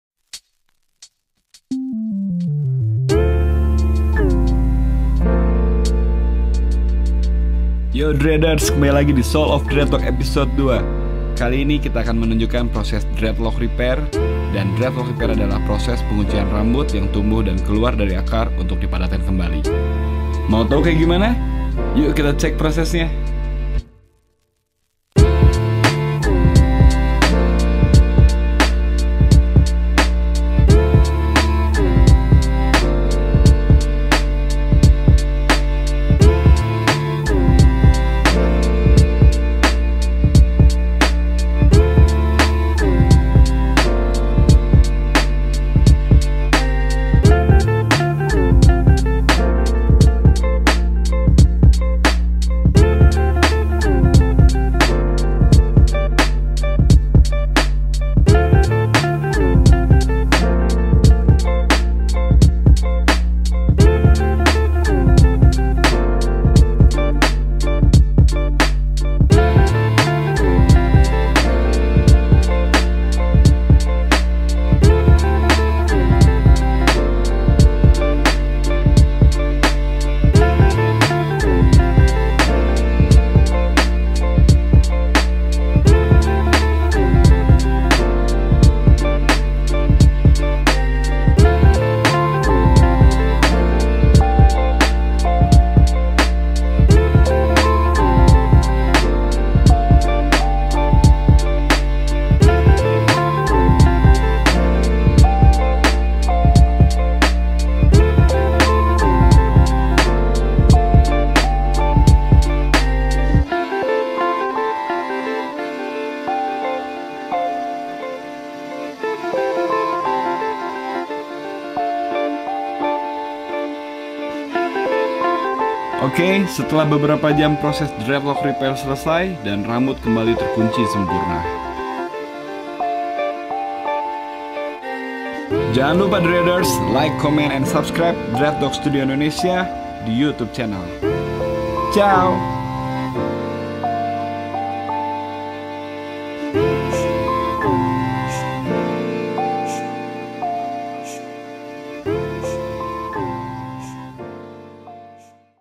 Rapihin Rambut Gimbal (Dreadlocks Repair)
Hi Dreaders!

Kali ini Dreadock Studio Indonesia akan memperlihatkan proses Dreadlock Repair, yaitu proses penguncian rambut yang tumbuh dari akar, agar rambut gimbal kembali rapih dan kencang seperti sediakala. Penasaran seperti apa prosesnya?

Dreadock Studio Indonesia
jl bukit dago selatan 1 no 4
Bandung - Indonesia